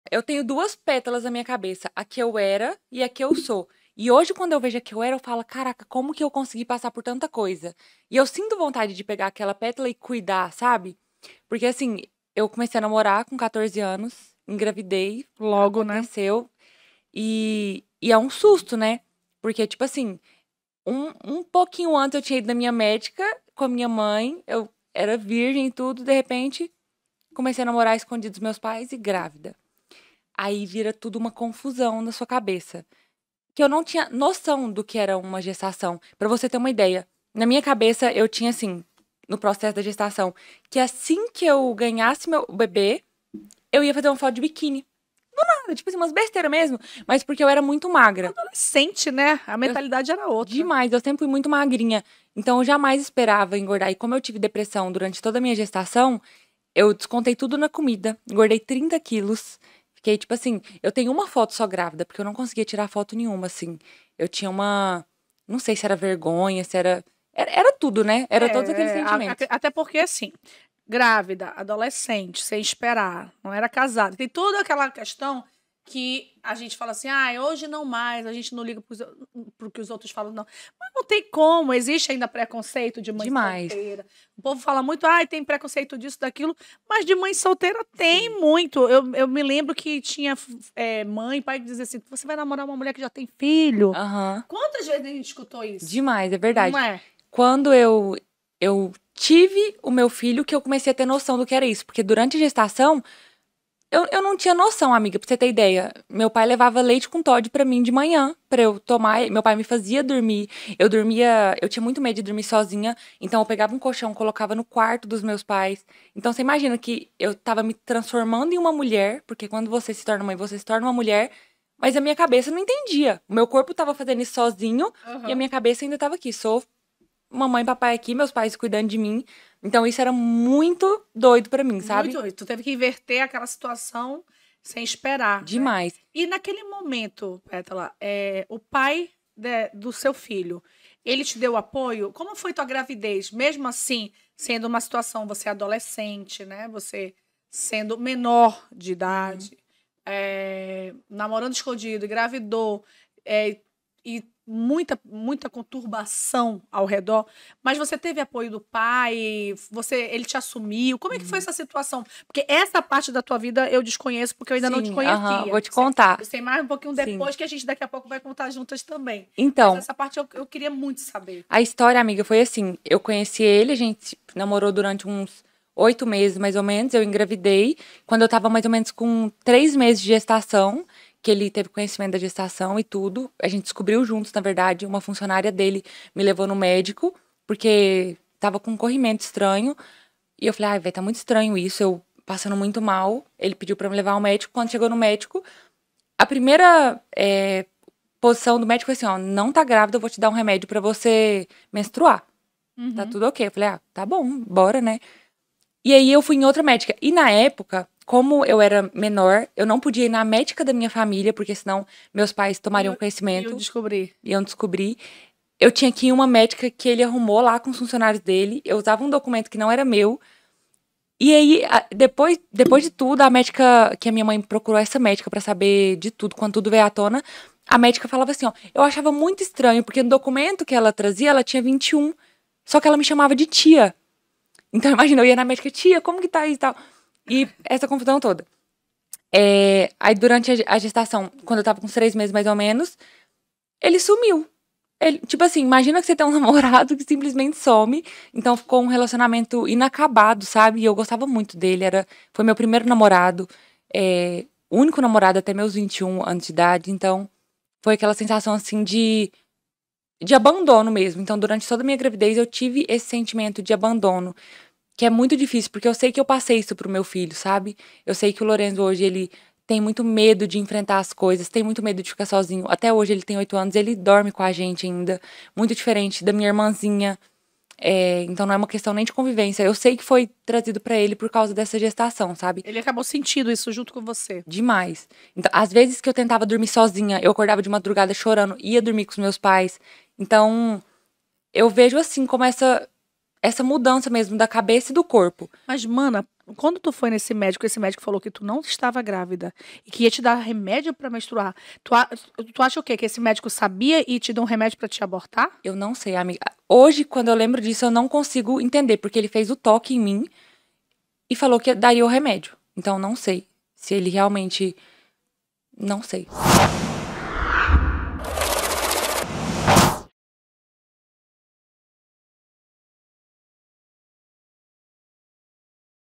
GRAVIDEZ AOS 14 ANOS | PÉTALA BARREIROS
Oi meus amores, tudo bem? É com muito orgulho que recebo a influenciadora e minha amiga, Pétala Barreiros no quadro Mulheres Fenomenais. Em um bate-papo pessoal, a Pétala desabafou e contou como foi sua gravidez aos 14 anos. Difícil né?

Acompanhe esse momento com a gente!

CONVIDADA
Pétala Barreiros: @petalagb